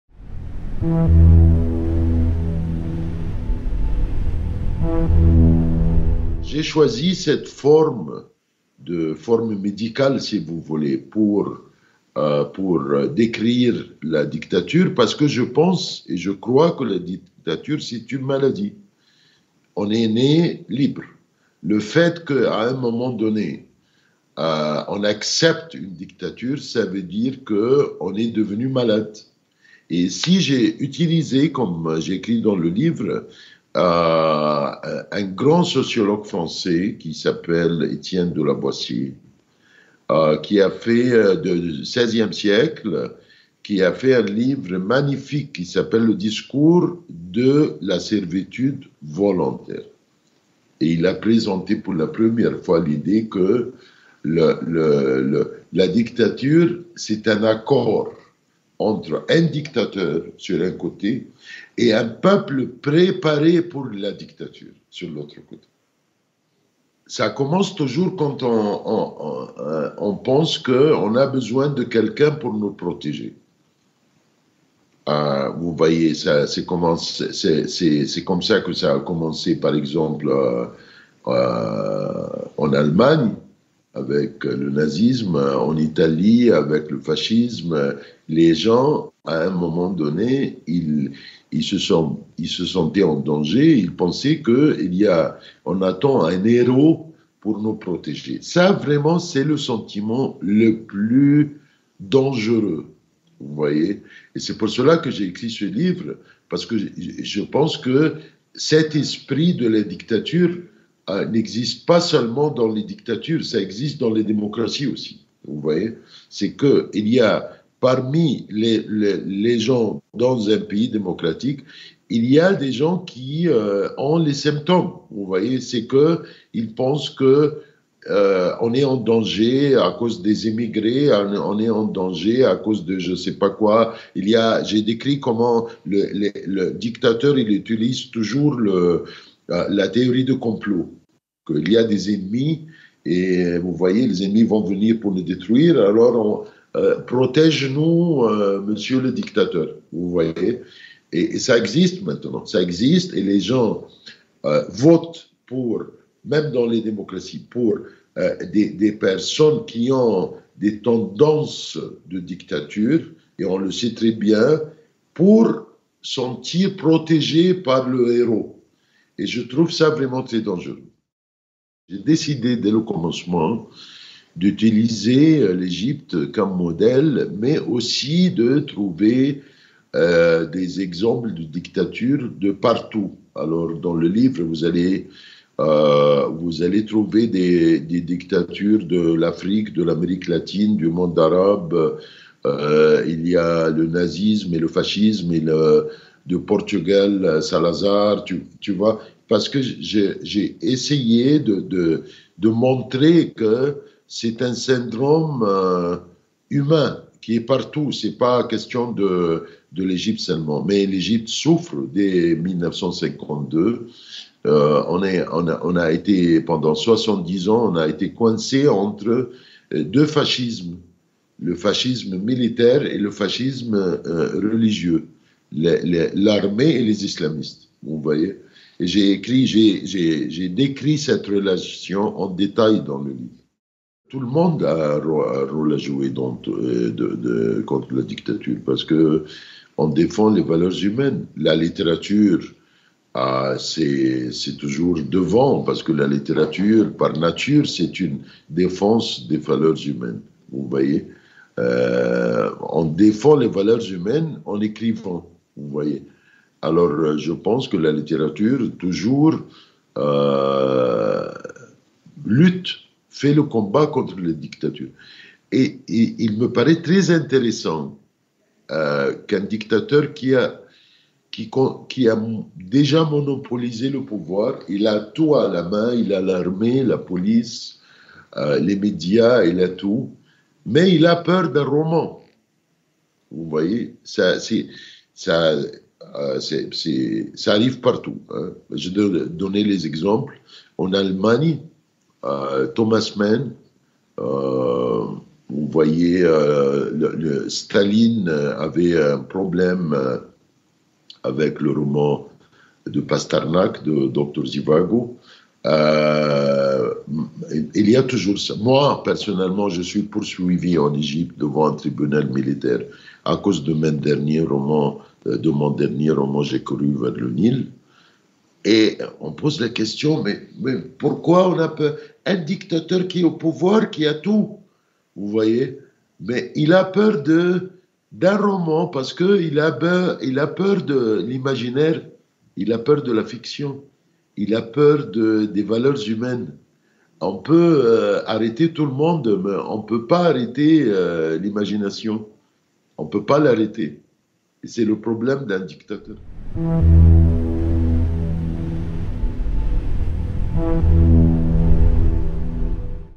Alaa el-Aswany - Le syndrome de la dictature
Alaa el-Aswany vous présente son ouvrage "Le syndrome de la dictature" aux éditions Actes Sud.